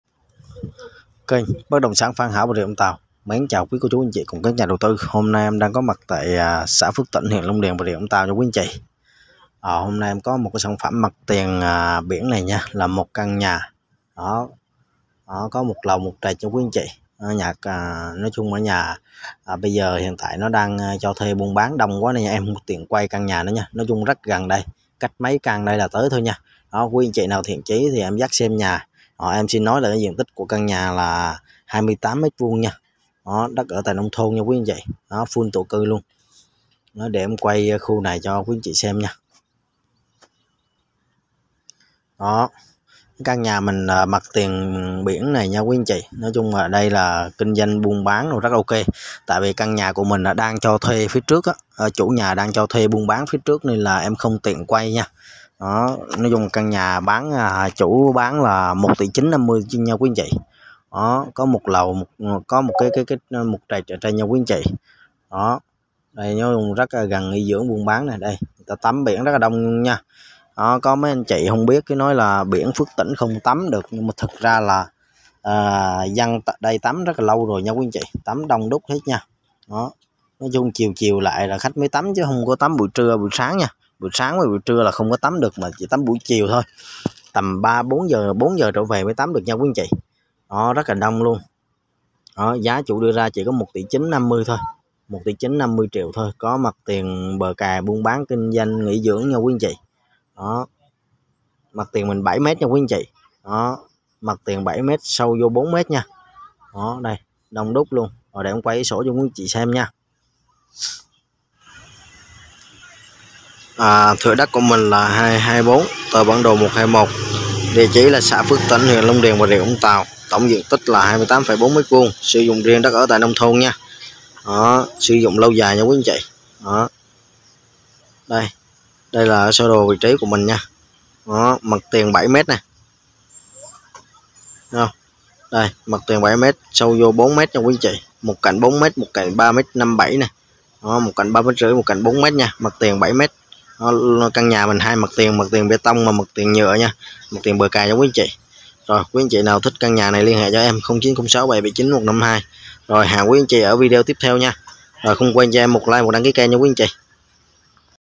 chủ gửi bán gấp căn nhà mặt tiền biển phước tỉnh. giá chỉ 1ty950tr.
anh chị nào quan tâm sản phẩm nha mặt tiền biển này alô e dắt xem nhà gặp trực tiếp chủ nhé. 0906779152